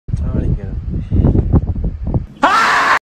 another marmot that screams like a man
This clip is an edit of an actual captured footage of a marmot screaming.   The sound of the scream is the one specifically edited.

Screaming Marmot part 2